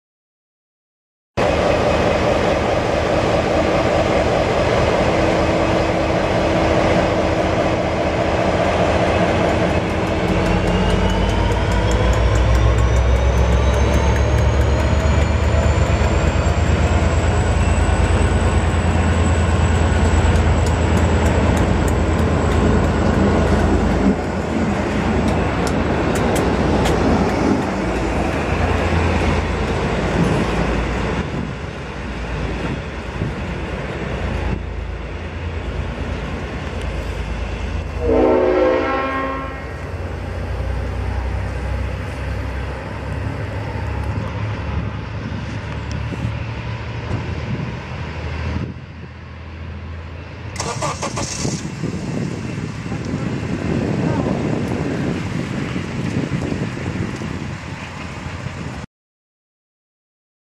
N472, Kerang, Victoria 3-6-2013
N472 with  Broad Gauge Melbourne Bound Swan Hill V/Line Passenger departing Kerang, Victoria on Monday 3rd June 2013

apologies for the shakiness as I didn't have a tripod with me, hope you'll like it nonethless.